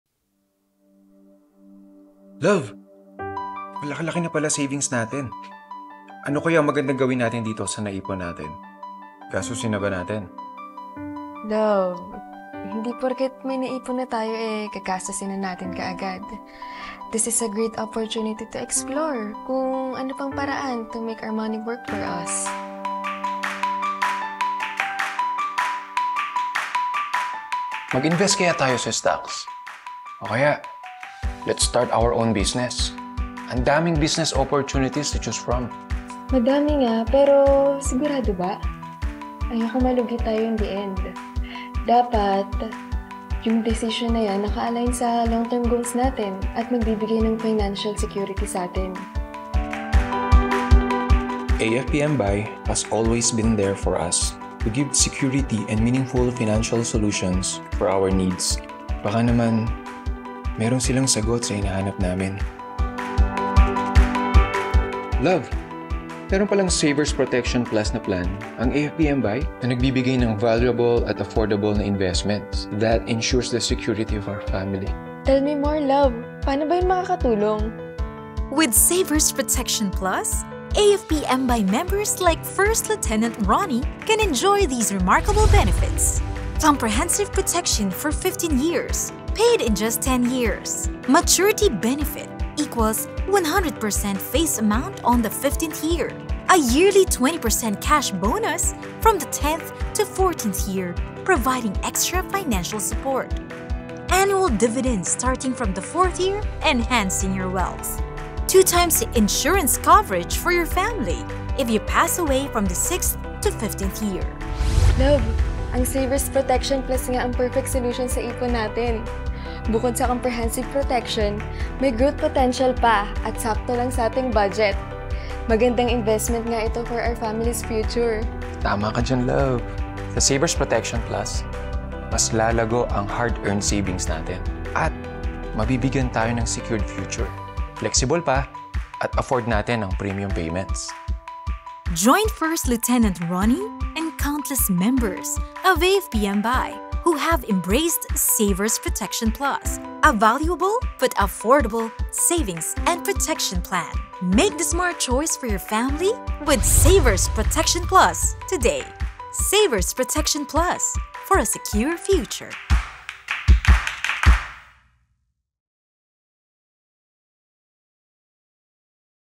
SAVERS PROTECTION PLUS
WHAT ARE ITS BENEFITS?

Valuable but affordable investment

1. Member is protected for 15 years which shall be paid in just 10 years
2. Dividends will be received on the policy’s 4th year which will continue annually until the year prior its maturity date
3. 2x the insurance amount will be given to the member’s family if the member passes away from the 6th-15th year of the policy
4. Additional 20% of the insurance amount will be received as a bonus starting from the policy’s 10th-14th year